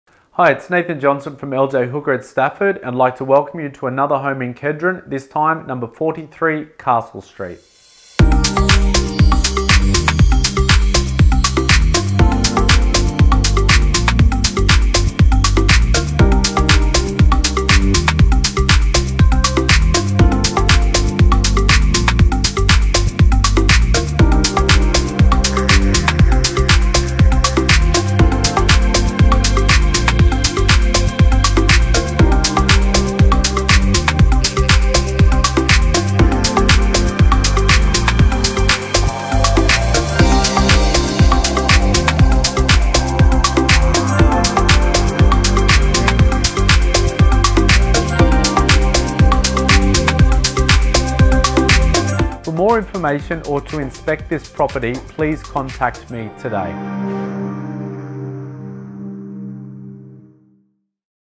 43 Castle St, Kedron
Located only 6.5km from the CBD and in the sought-after suburb of Kedron, this family home will surprise you with the space on offer. Upon entering you will be impressed with the decorative features that are present and the way the home opens up without compromising on privacy.

Walking through the home you will see there is little to do, just sit back and enjoy! With three bedrooms, one bathroom and a living area located in the front half of the house the kitchen provides separation to a parent retreat located at the back of the house. Here you will find another bathroom, bedroom and living area, both rooms open out through wooden French doors to a large covered deck. Fitted with café blinds this area will be a central part of your home all year round.

Features include:
Air-conditioned living area and main bedroom
Stone bench tops and walk in pantry
Remote controlled garage door
Crimsafe style security doors
Water tank
Fully fenced 580M² block
Plenty of storage

Residents of Kedron love the ease and location of this family-oriented community. Living here you are a short drive to the Padua Schools Precinct, 10 minutes' drive to the airport with convenient access to the tunnel network linking the Northside to the Gold Coast and Western suburbs.

The location offers a great lifestyle, being only a short 450m walk to the local Kedron State Primary School, 500m walk to Wavell State High School and 700m walk to sporting fields and acres of parkland and access to Kedron Brook bikeway that stretches for over 22km.
Don’t delay inspecting this family home, demand for this location is very strong.

Call Nathan for a full property report today.